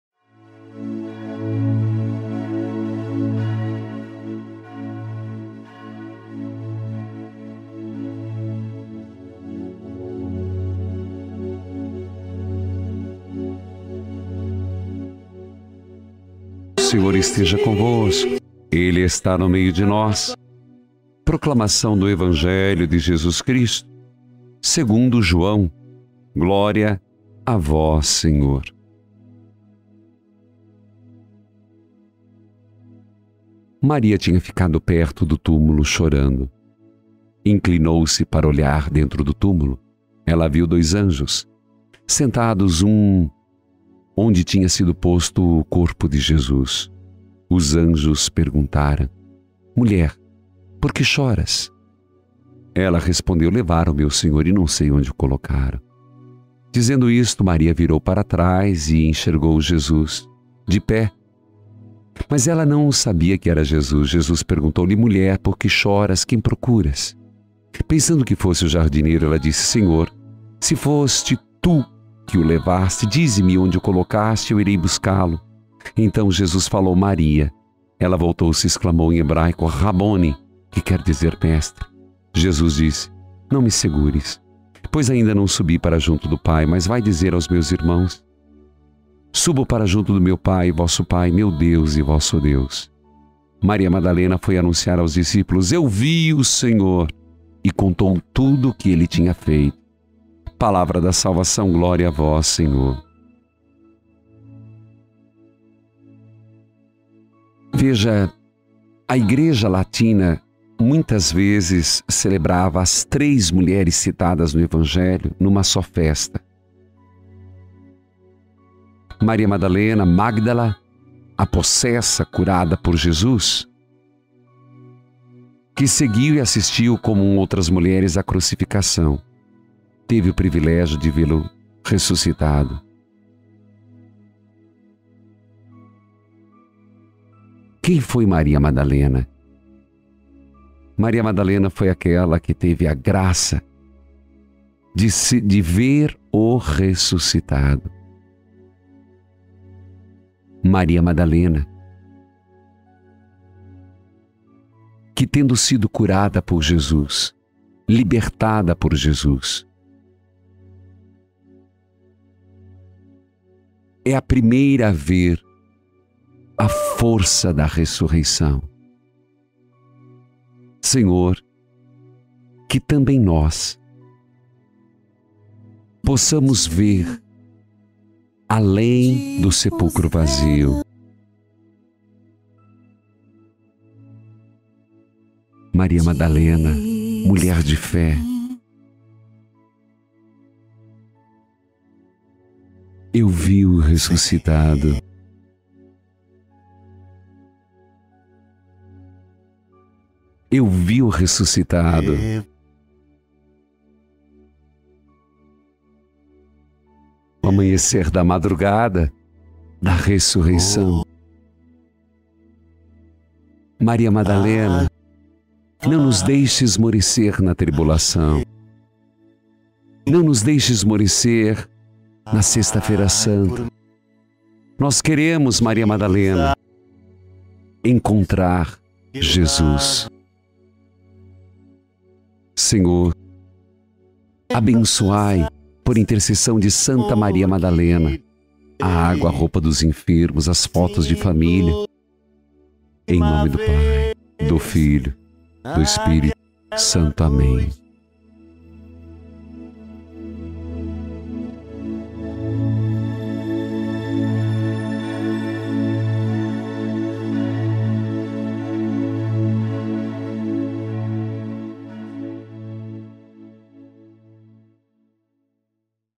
EVANGELHO DO DIA 22/07/2024 | Jo 20, 1-2 , 11 -18
A Leitura Orante de hoje é o Evangelho de Jesus Cristo segundo
 ( Jo 20, 1-2 , 11 -18 )

Acompanhe, diariamente, o Evangelho do Dia, 5h30, com Padre Reginaldo Manzotti

Junte-se a nós nessa jornada espiritual e fortaleça sua fé! 🌟🙌